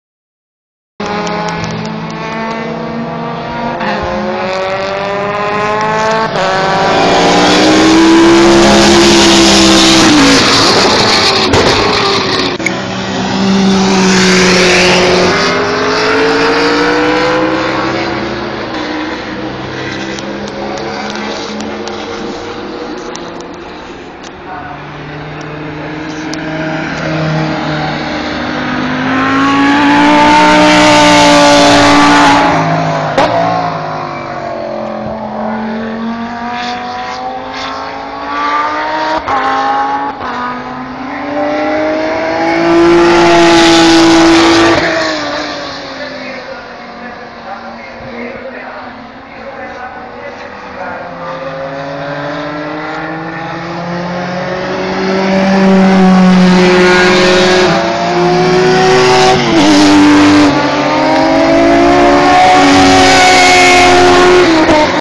24h of Zolder 2012 @ Corner : Eerste
I made this video at the 24 h of Zolder . I made this video whit my Cell phone , it not so good quality .